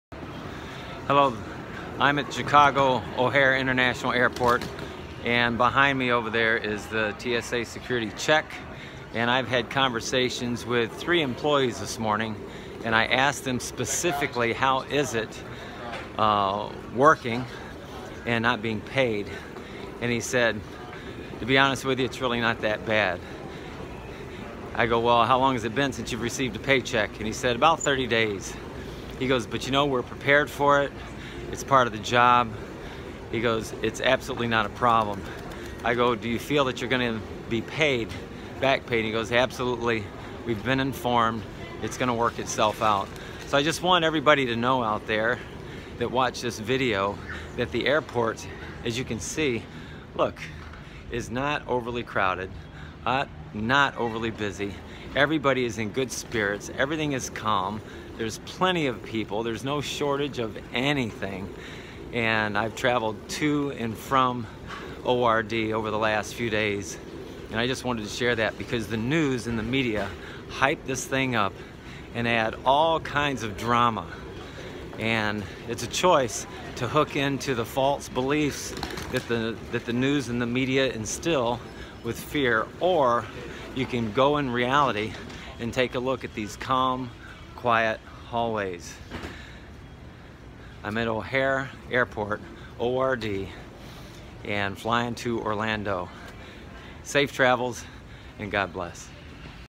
Government Shutdown at Chicago International Airport ORD January 22, 2019
Day 32 of no pay. See and hear for yourself how the shutdown affects the workers and travelers.